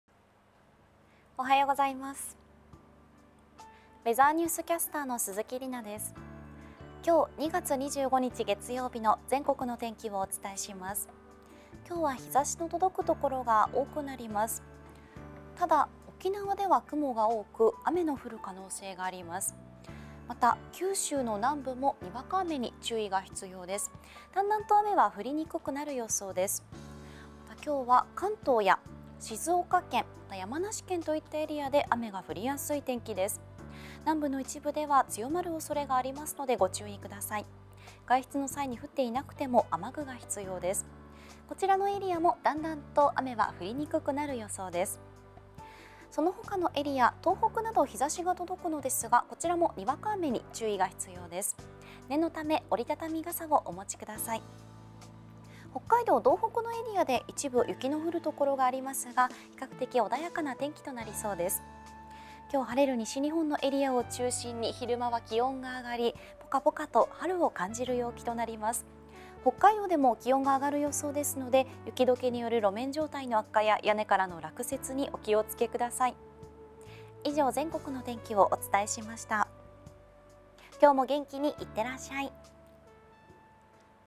お天気キャスター解説 2月25日(月)の天気
きょうは日差しの届くところが多くなります。西日本では春を感じるポカポカ陽気になりそうです。ただ、関東は雲が多く雨が降ります。一時的に強まるところもあるのでお気をつけください。東北も晴れるものの、にわか雨の可能性があるので折りたたみ傘があると良さそうです。

週明け月曜日ですね！今週の後半からはもう3月です。みなさん、3月の楽しみは何かありますか？今以上に春を感じられそうですね♪＾＾

それでは、きょうも元気にいってらっしゃい☆